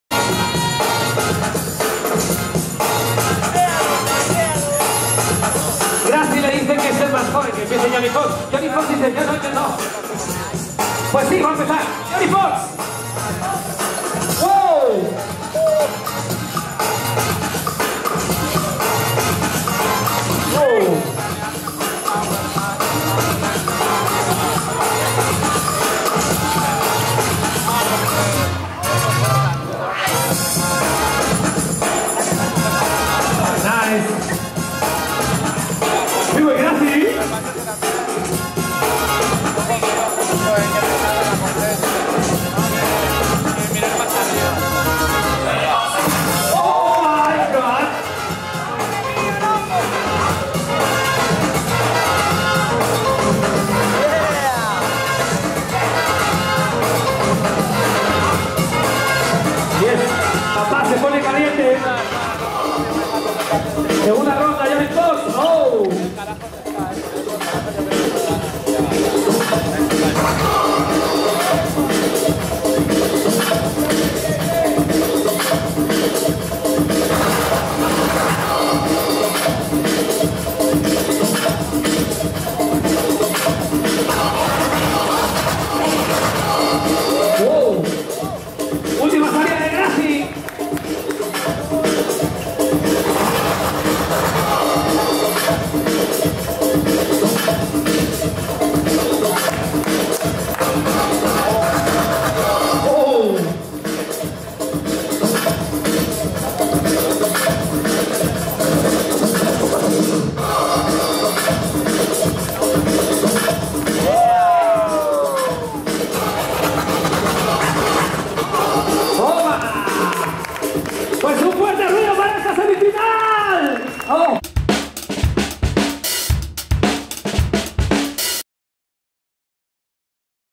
Johnny Fox vs Grazy | TOP 4 | Boty Iberica 2019 | OLIFILMS
Battle of the year Spain 2019
Video: Victor Gomez Alarcon

OLIFILMS TEAM.